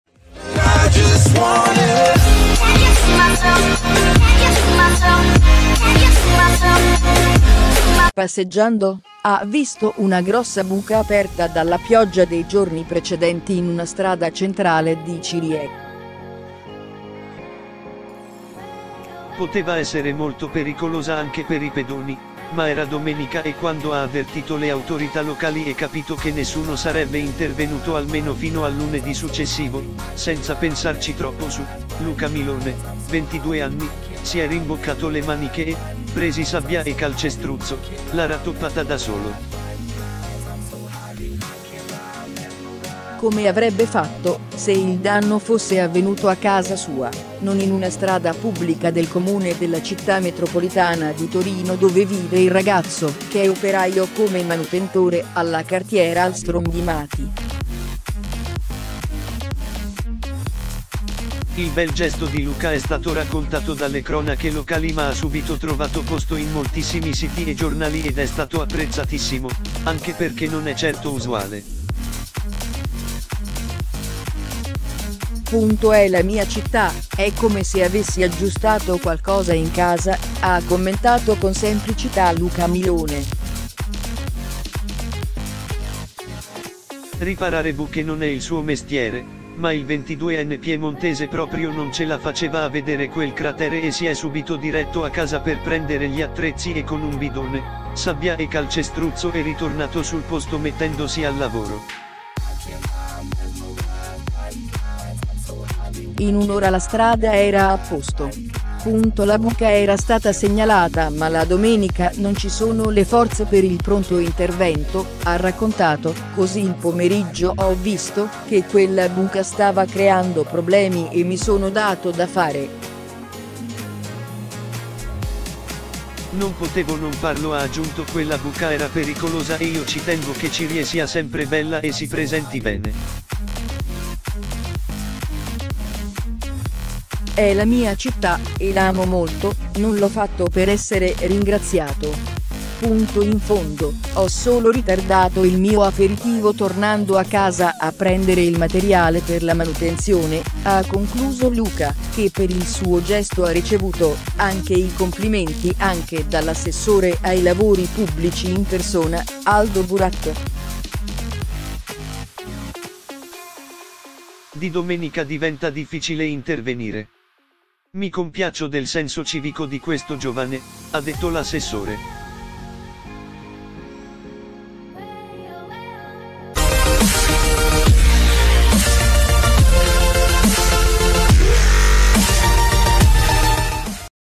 C’è una buca in strada, Luca (22 anni) corre a prendere gli attrezzi e la ripara
Passeggiando, ha visto una grossa buca aperta dalla pioggia dei giorni precedenti in una strada centrale di Ciriè. Poteva essere molto pericolosa anche per i pedoni, ma era domenica e quando ha avvertito le autorità locali e capito che nessuno sarebbe intervenuto almeno fino al lunedì successivo, senza pensarci troppo su, Luca Milone, 22 anni, si è rimboccato le maniche e, presi sabbia e calcestruzzo, l’ha rattoppata da solo.

Come avrebbe fatto se il danno fosse avvenuto a casa sua, non in una strada pubblica del comune della città metropolitana di Torino dove vive il ragazzo, che è operaio come manutentore alla cartiera Ahlstrom di Mathi. Il bel gesto di Luca è stato raccontato dalle cronache locali ma ha subito trovato posto in moltissimi siti e giornali ed è stato apprezzatissimo, anche perché non è certo usuale.
“È la mia città, è come se avessi aggiustato qualcosa in casa”, ha commentato con semplicità Luca Milone. Riparare buche non è il suo mestiere, ma il 22enne piemontese proprio non ce la faceva a vedere quel cratere e si è subito diretto a casa per prendere gli attrezzi e con un bidone, sabbia e calcestruzzo e ritornato sul posto mettendosi al lavoro. In un’ora la strada era a posto.
“La buca era stata segnalata ma la domenica non ci sono le forze per il pronto intervento – ha raccontato – Così il pomeriggio ho visto che quella buca stava creando problemi e mi sono dato da fare”. “Non potevo non farlo – ha aggiunto – Quella buca era pericolosa e io ci tengo che Ciriè sia sempre bella e si presenti bene. È la mia città e la amo molto, non l’ho fatto per essere ringraziato”.
“In fondo ho solo ritardato il mio aperitivo tornando a casa a prendere il materiale per la manutenzione”, ha concluso Luca, che per il suo gesto ha ricevuto anche i complimenti anche dall’assessore ai Lavori pubblici in persona, Aldo Buratto. “Di domenica diventa difficile intervenire. Mi compiaccio del senso civico di questo giovane”, ha detto l’assessore.